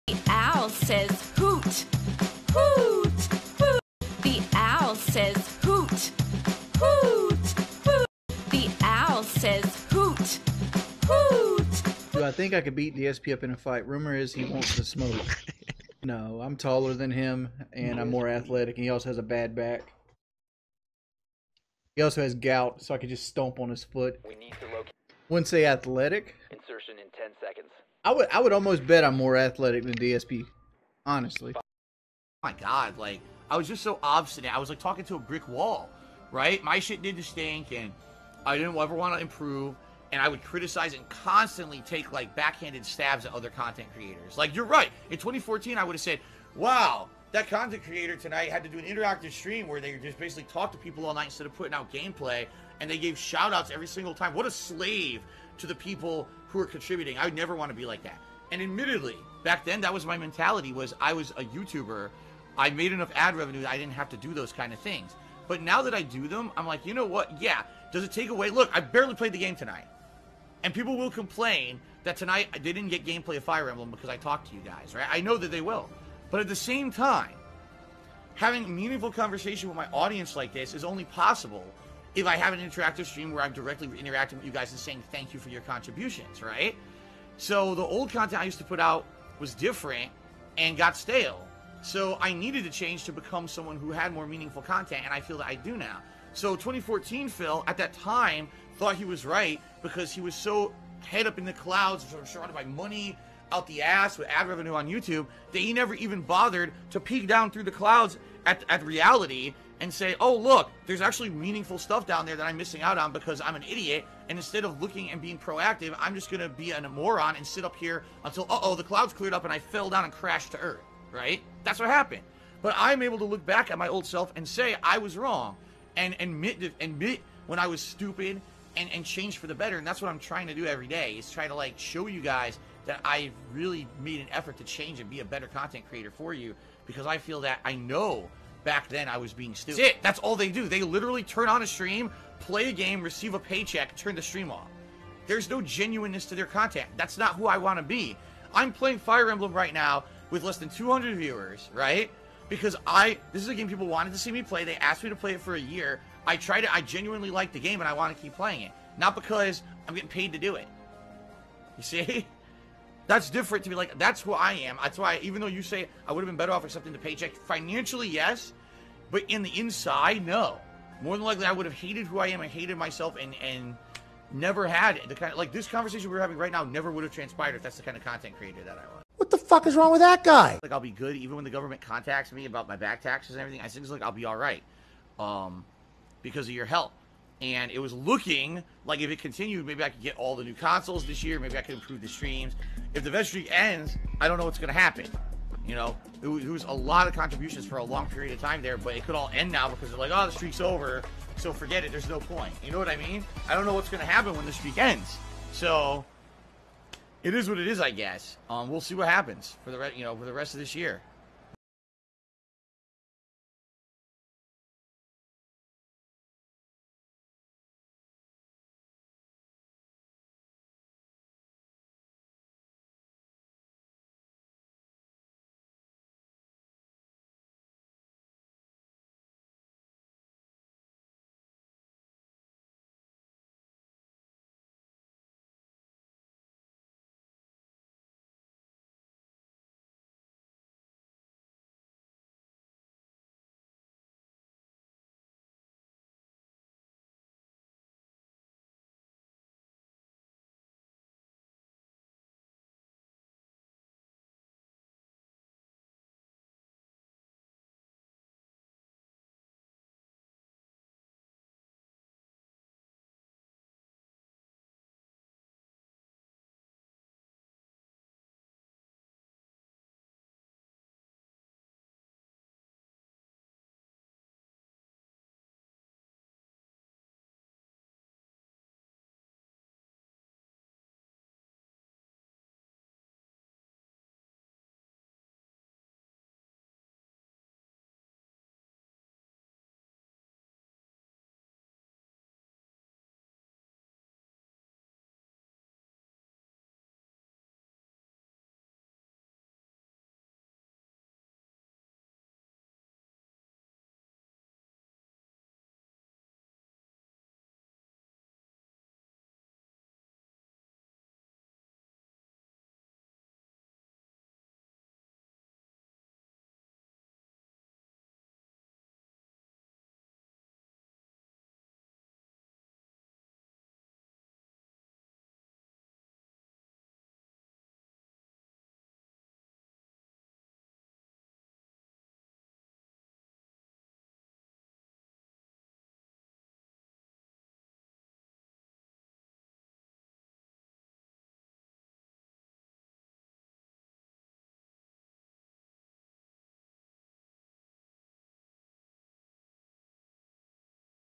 Wings Brags Can Beat DSP, Taxes, Micro transactions On Mobile Games, And Making Over $30 An Hour
DSP makes HOW much an hour? Someone did the math and... yikes!
I do love Wings says he can beat DSP because DSP has gout. Like man, when someone like Wings is probably right they can beat you in a fight, you have issues...

NOT CHILD FRIENDLY NOT MADE FOR CHILDREN!